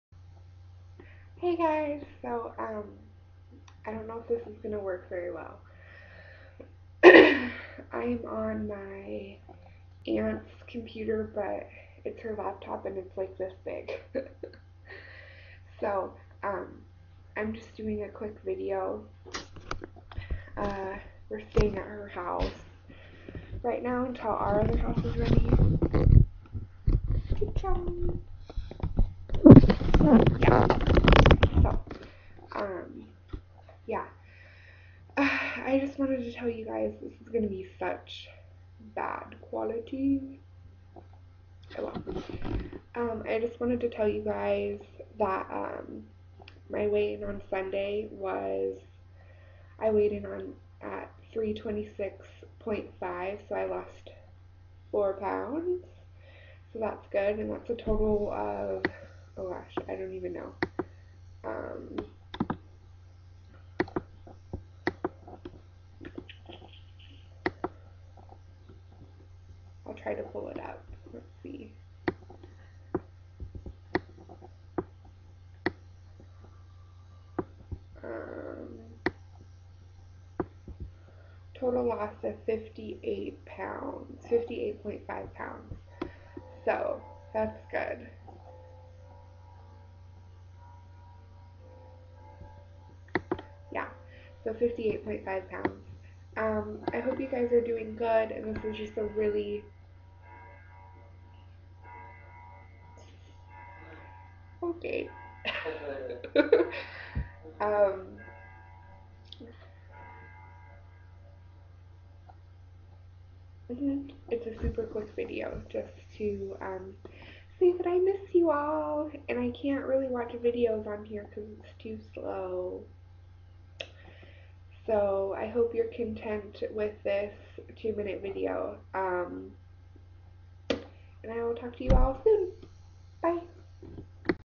Weight Watchers Weigh In Week 25
That channel is now designated only for vlogging and beauty related things. This channel will now be for all things weight loss related.

Started Weight Watchers 12/26/10 : 385 lbs.

Week 1 1/2/11 : 373 lbs. -12 lbs!
Week 2 1/9/11 : 370.6 lbs. -2.4 lbs
Week 3 1/16/11 : 371.3 lbs. +0.7 lbs
Week 4 1/23/11 : 367.7 lbs. -3.6 lbs
Week 5 1/30/11 : 367.7 lbs.
Week 6 2/6/11 : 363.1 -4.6 lbs.
Week 7 2/13/11 : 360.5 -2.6 lbs.
Week 8 2/20/11 : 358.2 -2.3 lbs.
Week 9 2/27/11 : 355.2 -3 lbs.
Week 10 3/6/11 : 353.2 -2 lbs.
Week 11 3/13/11 : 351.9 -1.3 lbs.
Week 12 3/20/11 : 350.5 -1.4 lbs.
Week 13 3/27/11 : 351.9 +1.4 lbs.
Week 14 4/3/11 : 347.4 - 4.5 lbs.
Week 15 4/10/11 : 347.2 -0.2 lbs.
Week 16 4/17/11 : 345 -2.2 lbs.
Week 17 4/24/11 : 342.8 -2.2 lbs.
Week 18 5/1/11 : 338 -4.8 lbs.
Week 19 5/8/11 : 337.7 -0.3 lbs.
Week 20 5/15/11 : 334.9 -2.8 lbs.
Week 21 5/22/11 : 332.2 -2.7 lbs.
Week 22 5/29/10 : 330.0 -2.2 lbs.
Week 23 6/5/11 : 330.2 +0.2 lbs.
Week 24 6/12/11 : 330.5 +0.3 lbs.
Week 25 6/19/11 : 326.5 -4 lbs.
Week 26 6/26/11 : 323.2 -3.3 lbs.
Week 27 7/3/11 : 327.4 +4.2 lbs.
Week 28 7/10/11 : 332.9 +5.5 lbs.
Week 29 7/17/11 : 328.9 -4 lbs.
Week 30 7/24/11 : NO WEIGH IN
Week 31 7/31/11 : 328.9 lbs.

Total Loss 89.1